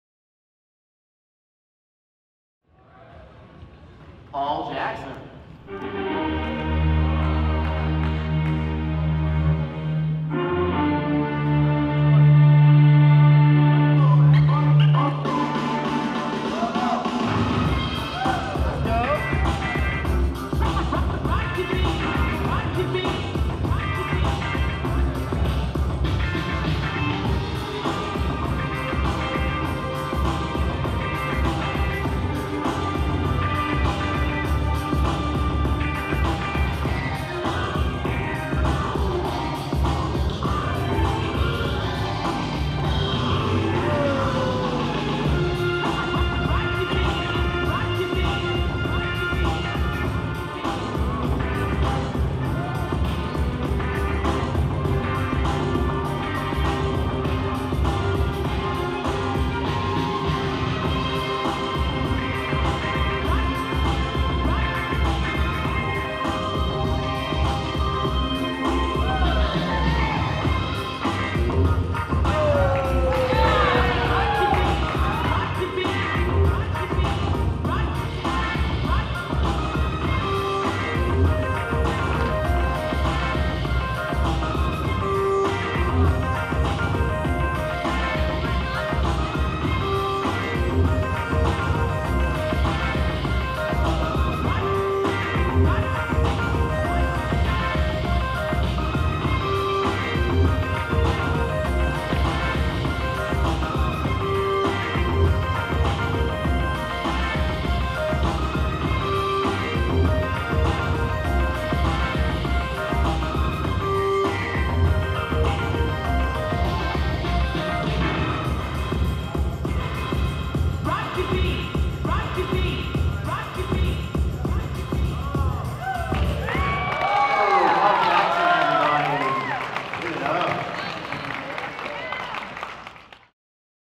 Paul Jackson - 1A Final - 6th Place - TX States 2016 - Presented by Yoyo Contest Central
The 2016 Texas State YoYo Contest was held on June 11th at the Bedford Boys Ranch Gymnasium in Bedford, TX. Presented by the National Yo-Yo League, this contest was made possible with support from many gracious sponsors
For full results and more information check out the official contest website and Facebook event

The social media hashtag is TXYYC